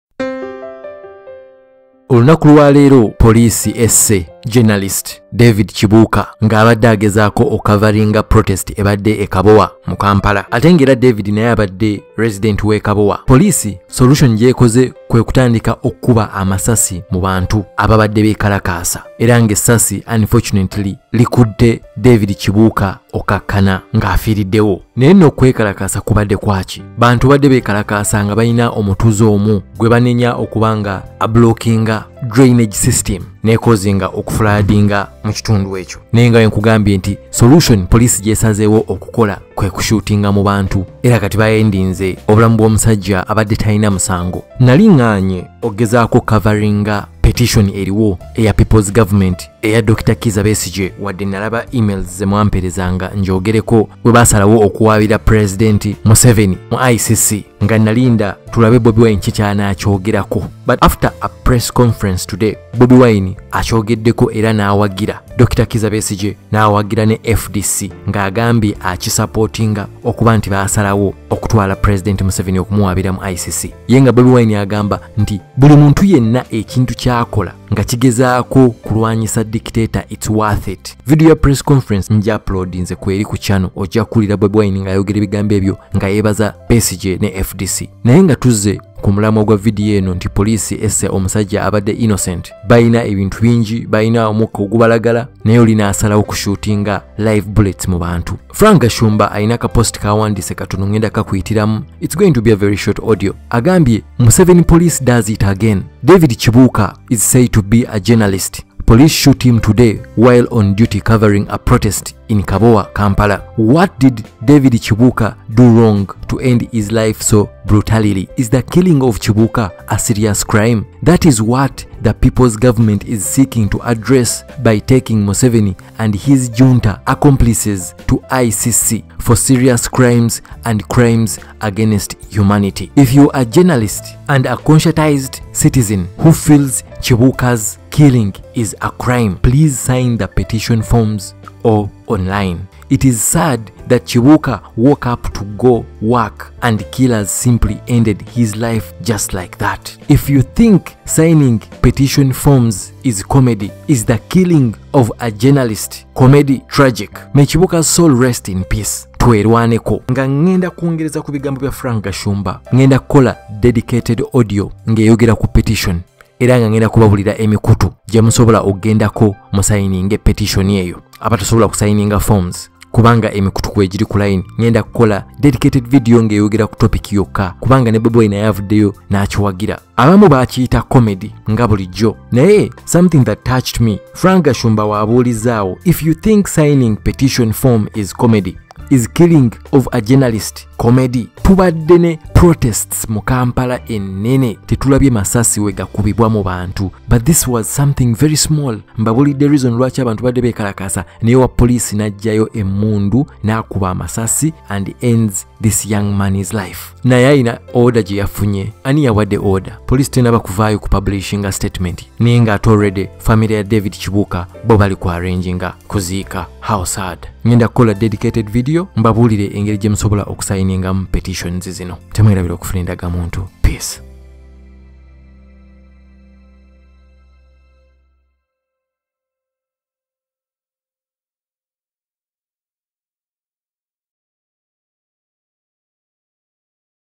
Police Esse Journalist Omulala David Kibuuka
David Kibuuka is said to be a journalist. Police eMUSE today while on duty covering a protest in Kabowa, Kampala.